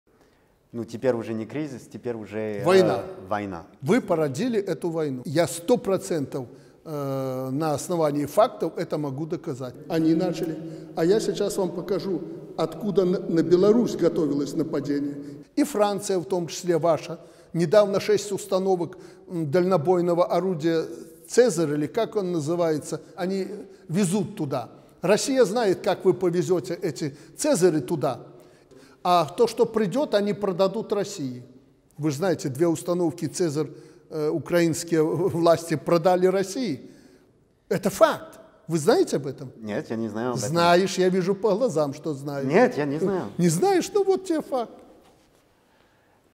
лукашенко-спекулянт придумал схему по продаже украинских сау "Caesar" в рашку!
21 июля 2022 года лукашенко вспомнил молодость и придумал очень выгодную схему по перепродаже поступающих в Украину из Франции сау "Caesar" рашке. А он, как известно, самый большой умелец по продажам краденого (в переводе на лукашенковский - "халявного"!)
Нет войне! Жыве БЕЛАРУСЬ!